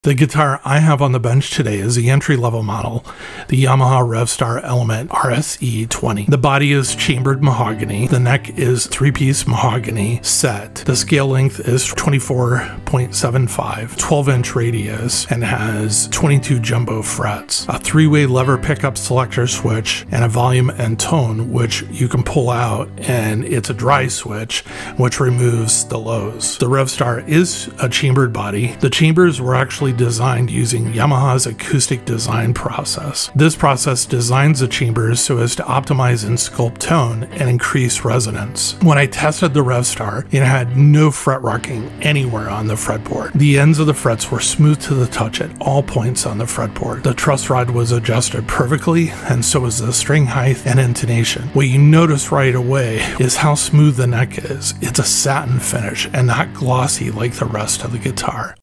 Yamaha Revstar Element RSE20 – On the Bench Today
I have been keeping my eye on the Yamaha Revstar II for a while now. This is the second iteration of the Yamaha Revstar. The online reviews are stellar. Almost too stellar. I have never see this much positive commentary on a guitar in a long while. While on Reverb.com I clicked on the watch heart on seven of the brand-new Element RSE20 and about 30 minutes later I had an offer for a brand new Yamaha Revstar Element RSE20 in Swift Blue with a cool racing stripe for $409. This was $90 off of the retail price listed.

There are three basic levels or lines of the Revstar guitar available to purchase. For the Standard and Professional lines there are sub-models and variations like humbucker pickups versus P90s or trem systems versus tune-o-matic.

The model number for the entry level is RSE20 (called the Rev Star Element). The Element would be sort of comparable to the Epiphone line or the higher end Squiers. I am sure in the comments people will argue with me on this. I am just trying to provide a point of price reference for contextual comparison. The Element models range new in price from $409 and up. Used they go for low $300s and up. The RSE20 model of the guitar is made in Indonesia.

Next is the Standard model which and the model numbers all start with RSS (Rev Star Standard). For the sake of this commentary, I would compare to the Fender player series or the Gibson Tribute or Studio models. This level of the Revstar goes for the low $700s to $849. This level of the guitar is also made in Indonesia.

Last is the Professional line and the part numbers all begin with RSP (Rev Star Professional) that is made in Japan and prices out at around $1999 and up depending on specifications.

The guitar I have on the bench today is the entry level model – Yamaha Revstar Element RSE20.

The specifications for this model are –

BODY: Chambered mahogany
NECK: 3-piece mahogany, set
SCALE LENGTH: 24.75”
NUT/WIDTH: Black synthetic/42mm
FINGERBOARD: Rosewood with pearloid dot inlays, 12” radius
FRETS: 22, jumbo
HARDWARE: Tune-o-matic-style bridge and stud tailpiece, die-cast enclosed tuners – nickel-plated
STRING SPACING, BRIDGE: 52.5mm
ELECTRONICS: 2x YGD-designed VH3 covered humbuckers, 3-way lever pickup selector switch, volume and tone (with pull-push Dry Switch high‑pass filter)
WEIGHT (kg/lb 7.74lbs
RANGE OPTIONS: See RSS20 and RSP20
FINISHES: Neon Yellow, Black, Red Copper, Swift Blue (my guitar), Vintage White – gloss polyurethane body with satin neck back

To say that the body is chambered does a disservice to the Revstar. Yes, the chambered body does help reduce the weight of the body but they also have another function. The chambers were designed using Yamaha’s Acoustic Design Process. This process designs the chambers so as to optimize and sculpt tone and increased resonance. The Standard and Professional models have carbon reinforcement in the necks and the body to help improve vibration transfer. The Element model does not.

Here is what I will say about the element. Playing the guitar not plugged in it has the best sounding tone and resonance of any solid body electric guitar I have ever played. Even though the Revstar body has chambering it does not have sound holes, so it is considered a solid body and not a hollow body guitar. Plug in the Revstar element and it only improves. Even on my Positive Grid small bench amp it sounds incredible.

Next, let’s talk about the out of the box experience. The other day I took a brand guitar out of the box directly from the factory. I needed to adjust the truss rod, string height, and intonation to make the guitar what I would call playable. It was a high-quality guitar. When I examined the guitar there was fret rocking at three places on the fret board and the ends of the frets were about on a scale of 1 to 10 were a six. Meaning they were playable but a little rough in places but very playable. They needed attention.

When I tested the Revstar it had no fret rocking - anywhere. I ran my hands several times up and down the sides of the fret board. The ends of the frets were smooth to the touch at all points on the fretboard. The fret board did not need oiling before putting a new set of strings on. The truss rod was adjusted perfectly and so was the string height and intonation. I plugged the guitar in, and everything functioned exactly the way it was supposed to. The guitar finish is beautiful and unique with a glossy finish. What you notice right away is how smooth the neck is. It is a satin finish and not glossy like the rest of the guitar. Amazingly playable guitar.

There is nothing I would replace or upgrade on this guitar. I would not characterize this guitar as being a mod platform. It just does not need it.

In the past six months I have unboxed a lot of guitars. I would characterize this unboxing as the best experience I have had in a very long while. I am very impressed.